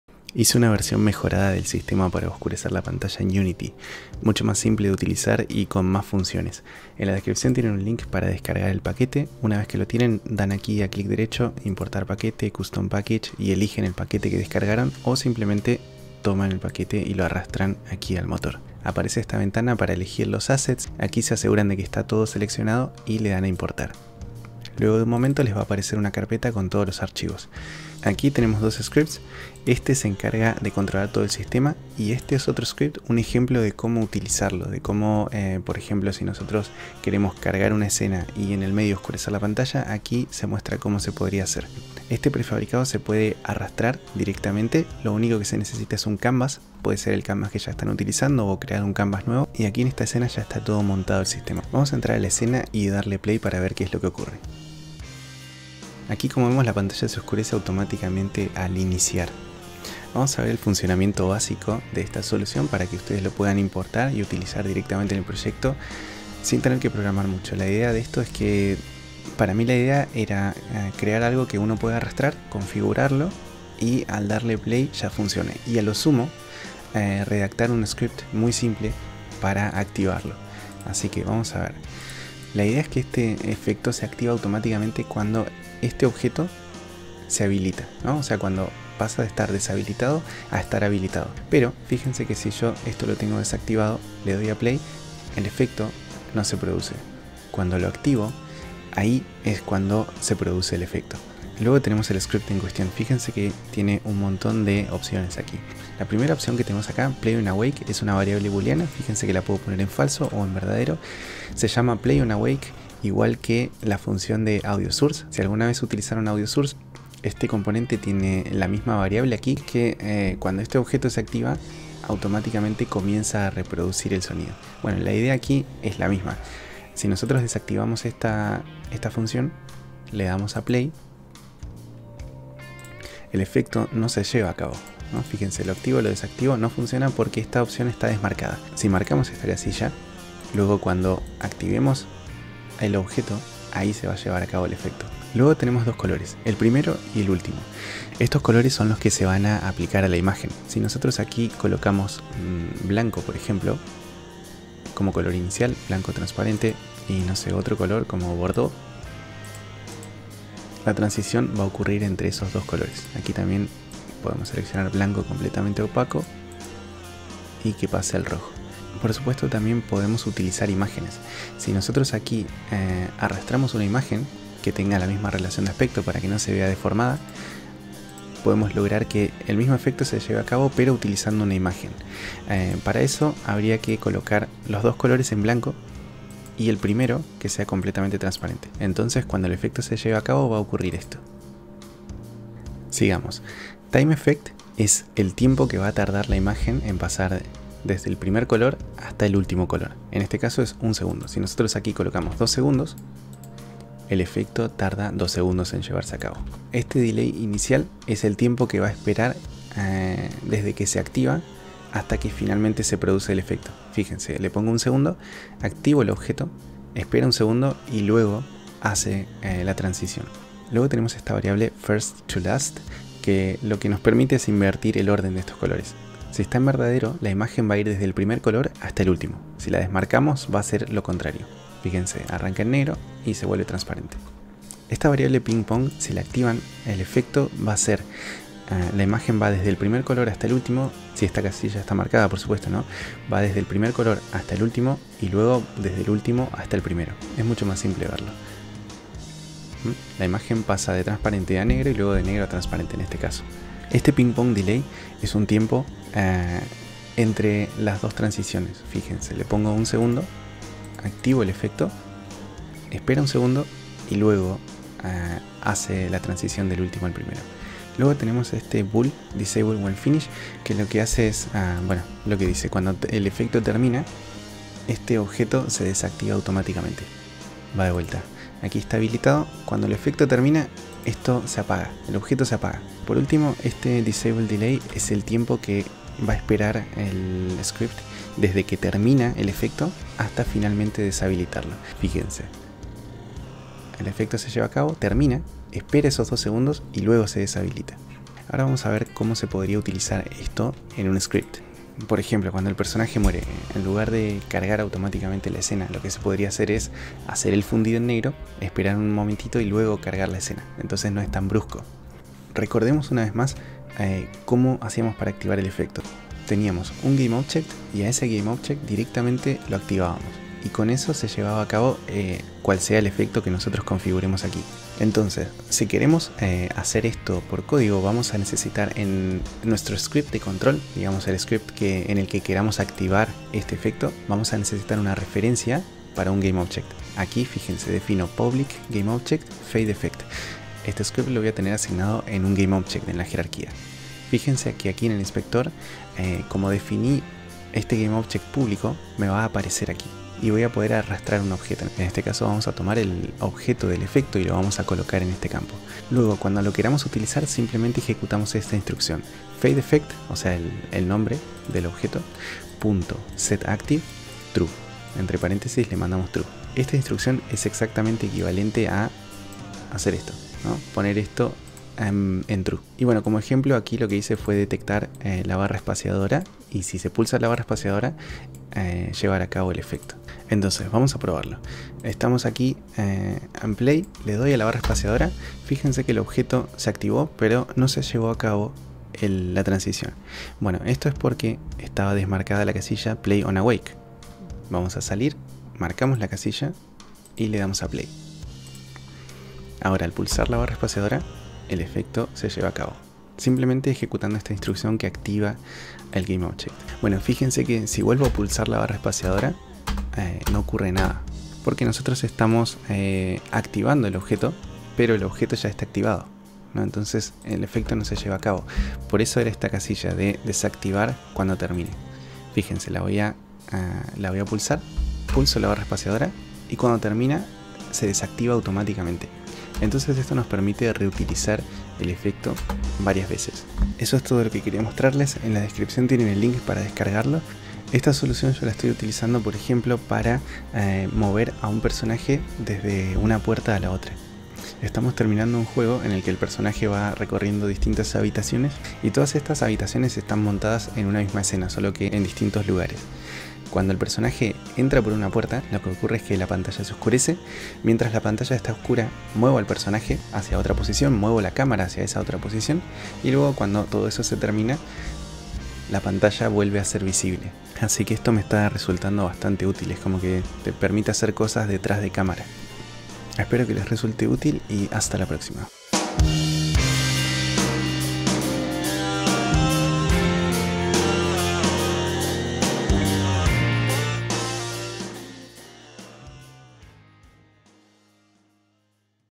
{ Efecto de TRANSICIÓN fundido de imagen PERSONALIZABLE para Unity } - Paquete Unity 📎
He creado una solución para el efecto de oscurecer la pantalla en Unity, mucho completa y simple de utilizar, que puedes descargar, importar y arrastrar al Canvas de tu proyecto para usarla. La idea era hacer un prefabricado que tenga muchas opciones para lograr distintos efectos, desde un efecto Fade in/Fade out o fundido en negro, en el cual la cámara se oscurece o se vuelve visible, hasta personalizar los colores del fundido, la velocidad con la que se produce o incluso utilizar imágenes.
Este efecto sirve por ejemplo para hacer transiciones de cámara, en mi caso lo estoy usando en un proyecto en el que el personaje al entrar por una puerta, se traslada a otro punto de la escena y para disimular el efecto, hago un fundido en negro, muevo el personaje y luego hago la pantalla visible nuevamente.

*LINKS*